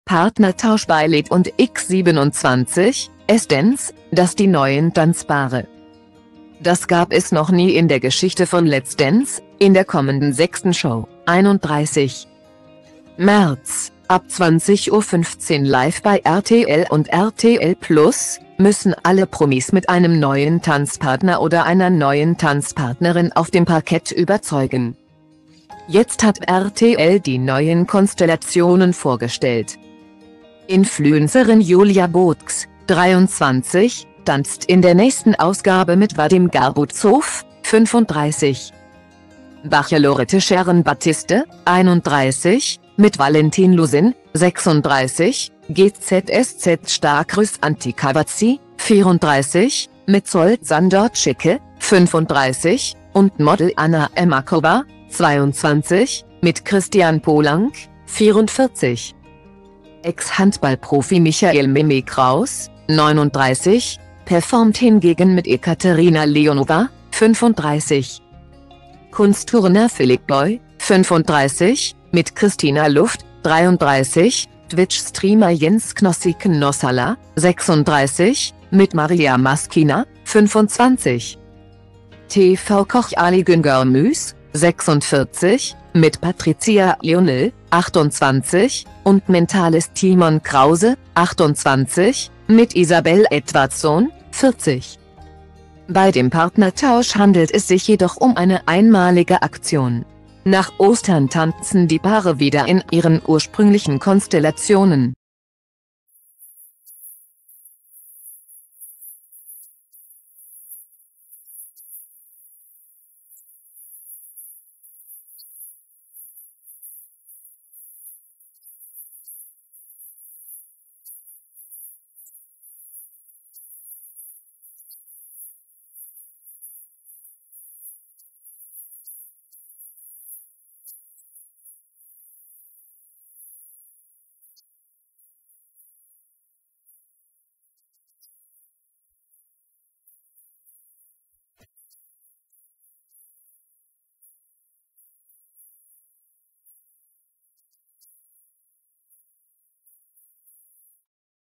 Partnertausch bei "Let&x27;s Dance": Das die neuen Tanzpaare
Das gab es noch nie in der Geschichte von "Let's Dance": In der kommenden sechsten Show (31.März, ab 20:15 Uhr live bei RTL und RTL+) müssen alle Promis mit einem neuen Tanzpartner oder einer neuen Tanzpartnerin auf dem Parkett überzeugen.Jetzt hat RTL die neuen Konstellationen vorgestellt.